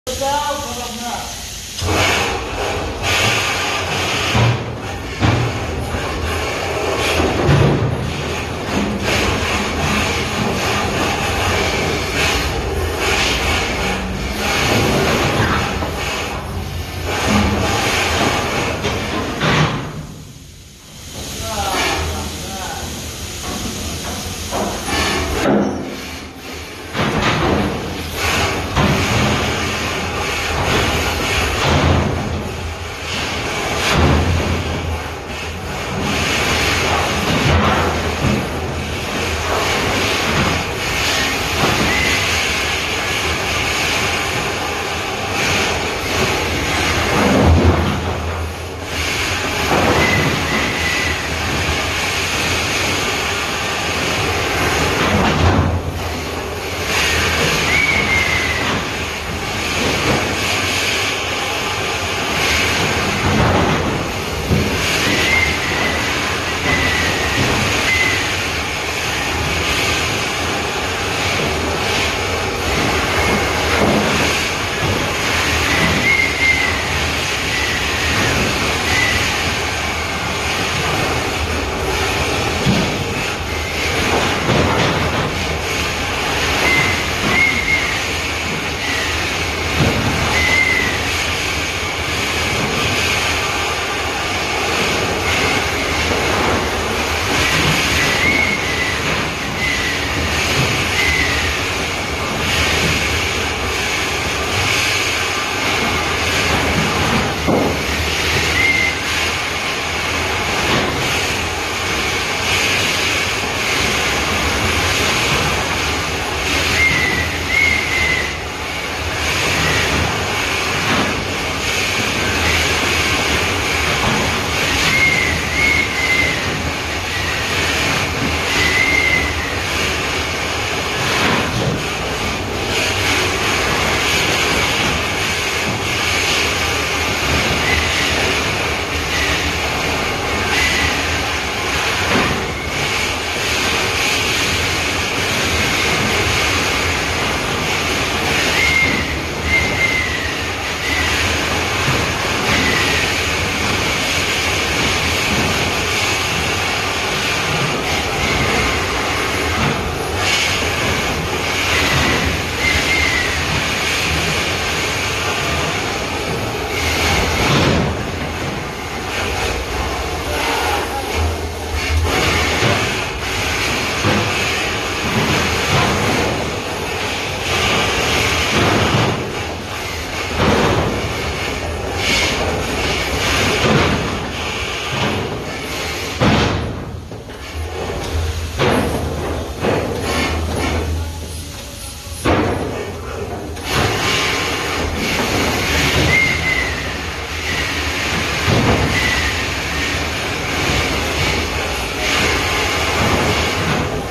Shaft Mucking, Underground Mining
Mucking out during shaft sinking in Northern Ontario, Canada 🇨🇦